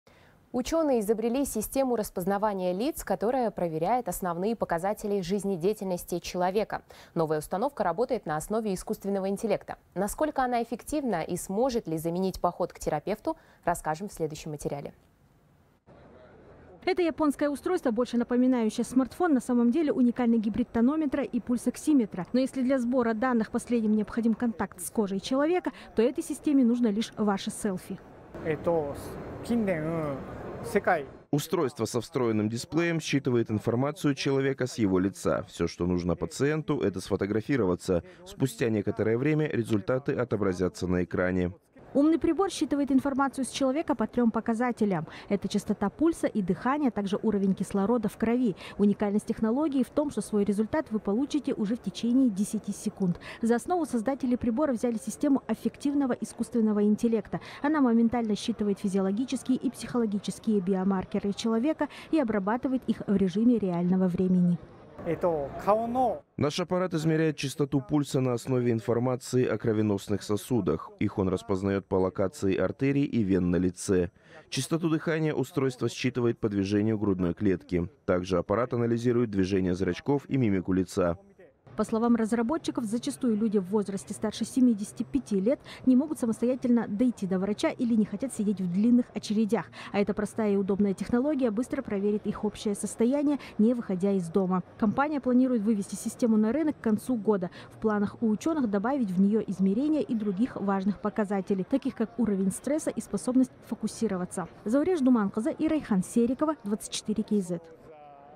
Учёные изобрели систему распознавания лиц, заменяющую поход к врачу
Ученые изобрели систему распознавания лиц, которая проверяет основные показатели жизнедеятельности человека. Новая установка работает на основе искусственного интеллекта. Насколько она эффективна и сможет ли заменить поход к терапевту, узнавал корреспондент «24KZ».